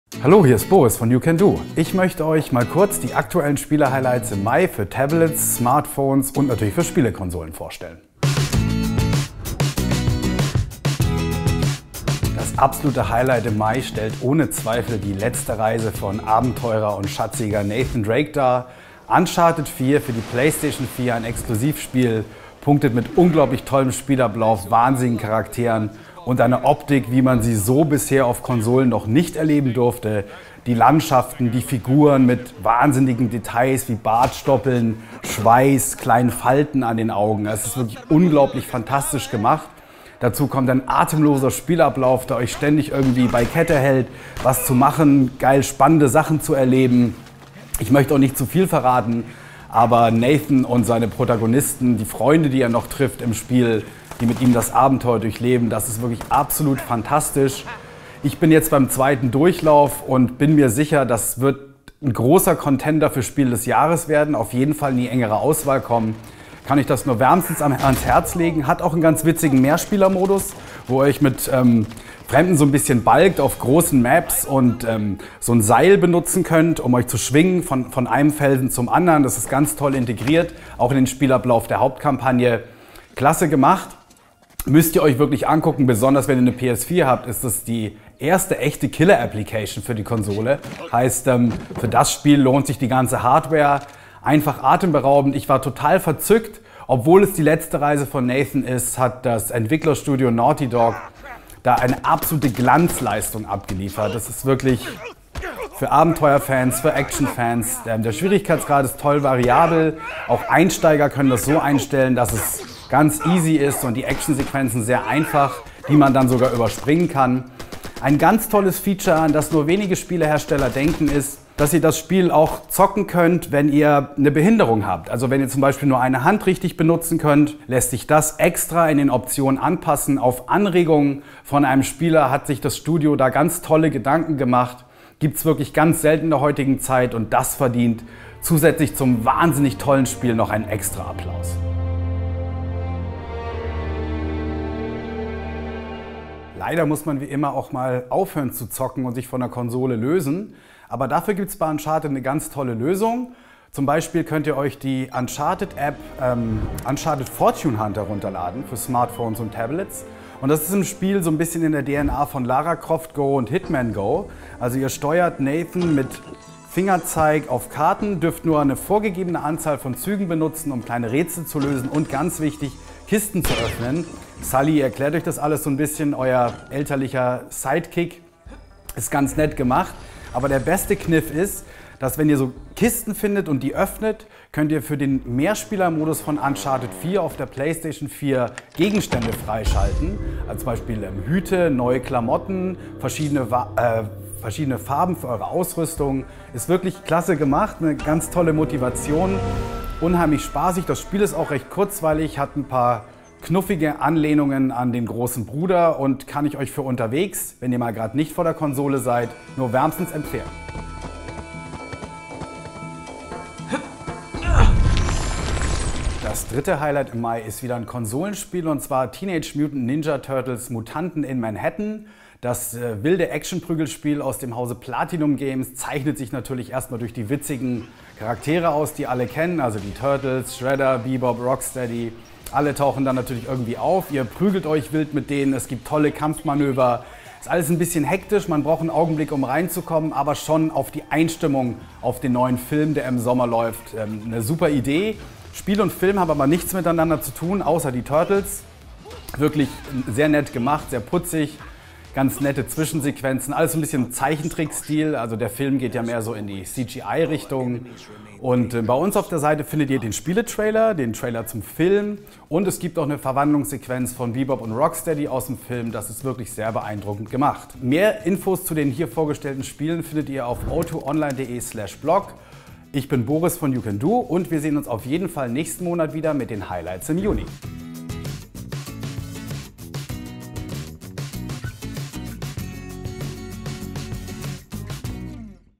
Die besten Spiele für Konsolen und Smartphones im Mai
YouCanDo-Redakteur stellt euch seine persönlichen Spiele-Highlights für Konsolen und Smartphones im Monat Mai vor.